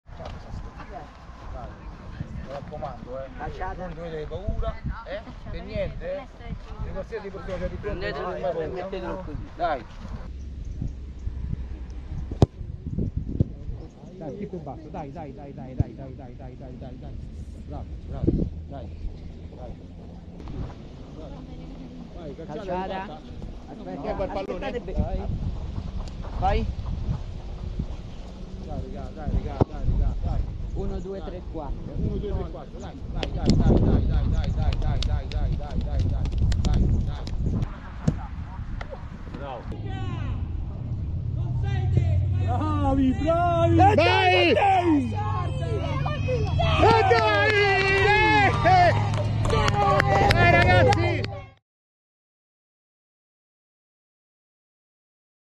UNDER 14 STAGIONE 2022/2023 | Atl Zagarolo-Guidonia 4-2
Prima giornata di campionato per l’Under 14 nel campionato 2022/2023. Risultato finale: Atletico Zagarolo-Guidonia 4-2.
📼 Godetevi il video con alcuni momenti della prima giornata e anche prima partita ufficiale agonistica per questi ragazzi classe 2009.